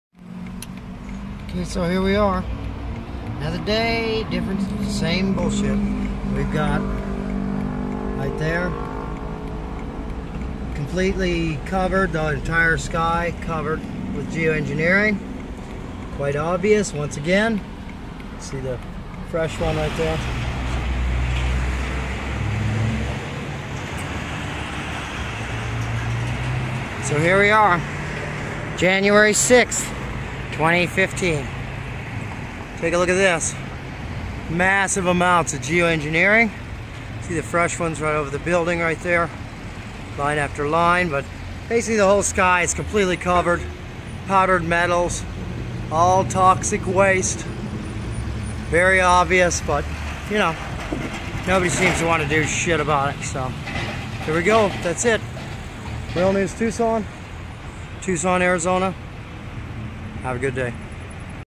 Downtown Tucson Az. covered with Toxic Waste! Tucson weather 01/06/2015
01/06/2015 Weather modification called Stratospheric Geo-Engineering or Chemtrails is being conducted again over the city of Tucson Arizona by using large jet airplanes as tanker sprayers! Even though it is quite obvious to anyone who has an IQ level at or above an average 5 year old child, the people residing in Tucson Az. do not stand up for themselves and they allow airplanes to spray toxic waste into the air they breath on a daily basis! Just like the fluoride poison which was first documented being used against the people which the Nazi's rounded up and put into camps before they exterminate them. The people of America allow their own government to poison them by adding fluoride to their drinking water even though it does nothing to help prevent cavities in their teeth but rather causes a disease in their gums! The effect of Poisoning our food(GMO's +), water(Flouride) and air(Geo-Engineering) is that the citizens do not stand up for themselves even though they are basically just rats in a laboratory being experimented on and exterminated!
Folks, DO NOT LOOK UP, DO NOT QUESTION YOUR GOVERNMENT! PAY YOUR TAXES AND MOVE ALONG! NOTHING TO SEE HERE!
 Real News Tucson